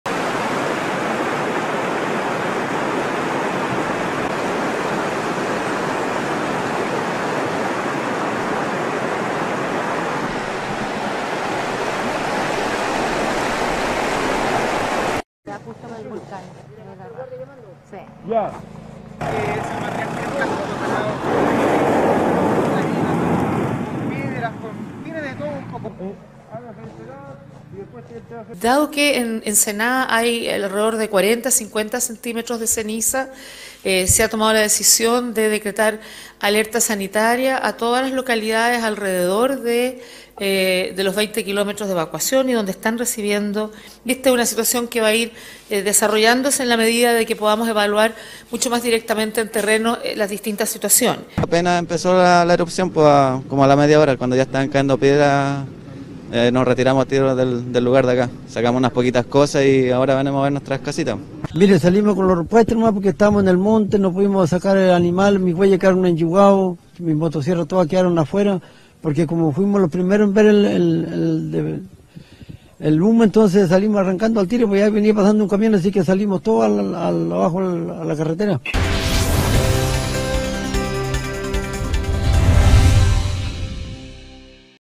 Bachelet anunció el estado de alerta sanitaria en los alrededores del Volcán Calbuco
La presidenta de Chile anunció el estado de alerta sanitaria para toda la zona de influencia del Volcán Calbuco, cuya erupción obligó a los pobladores del lugar a abandonar sus respectivas viviendas.
En conferencia de prensa, luego de una reunión con funcionarios locales, la Mandataria explicó que dicha decisión fue adoptada ante la presencia de alrededor de veinte centímetros de cenizas en la región.
Los efectos del Calbuco también se está haciendo sentir en varios sectores de la Patagonia argentina, como por ejemplo en el Cerro Catedral, en donde se suspendieron todas las actividades.